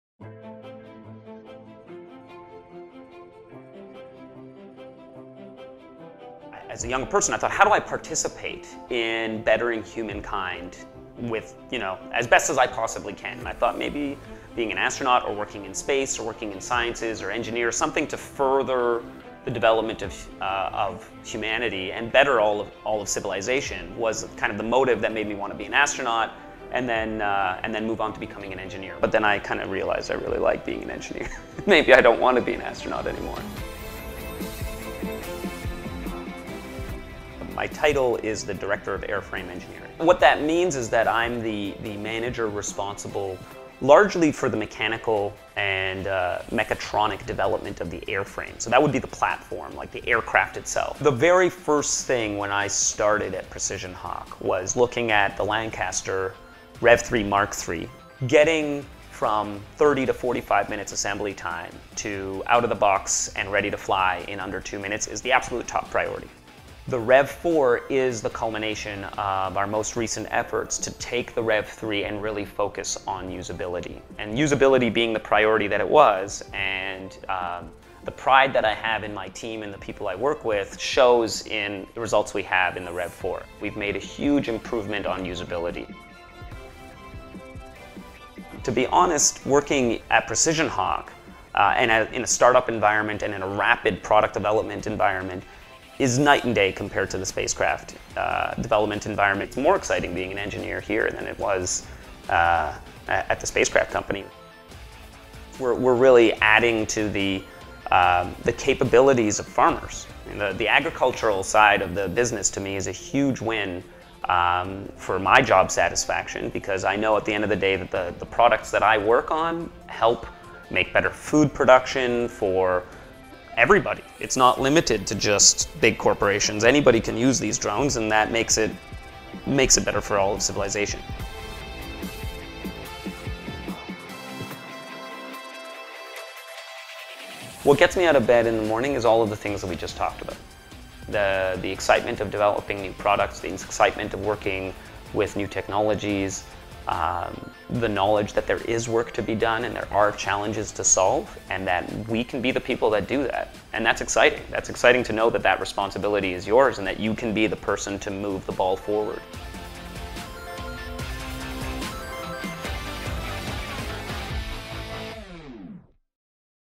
A Bird's Eye View: Meet Adam Latour
'A Bird's Eye View' - A snapshot of the team behind the PrecisionHawk Technology.

Adam Latour is Director of Airframe Engineering for PrecisionHawk. For more on Adam or to get information on joining our team,